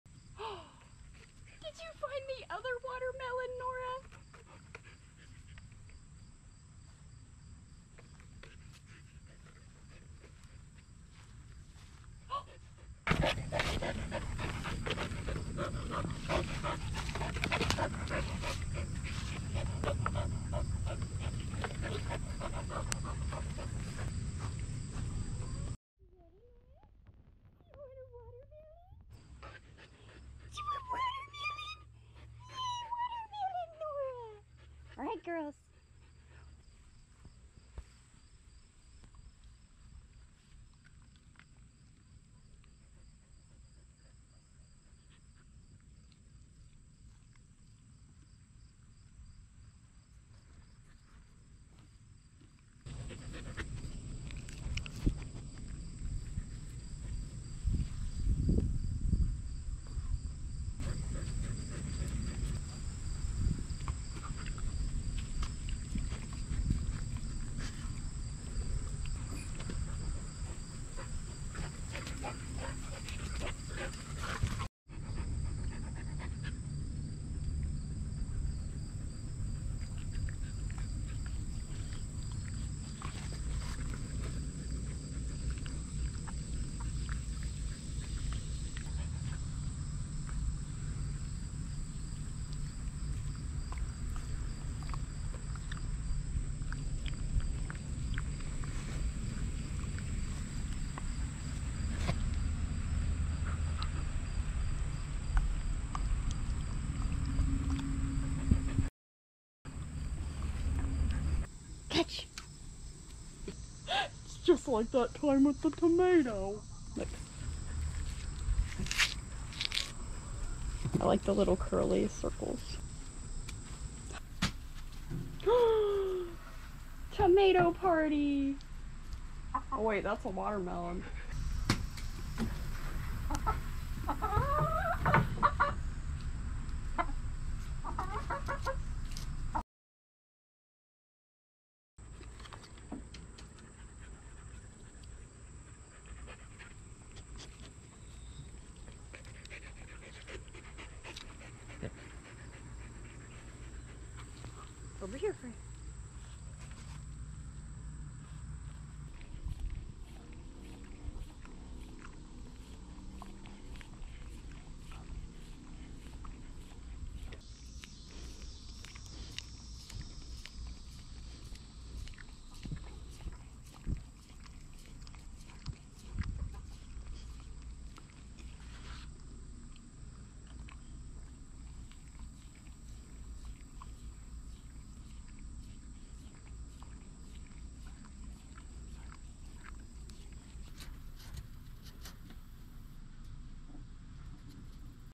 HUSKY VS WATERMELON! 🍉🐺
Nora the husky loves to try new food! 🍉 Let's see how she and her sister do versus a watermelon ! And of course, some puppy ice cream as a reward. 🐺🍦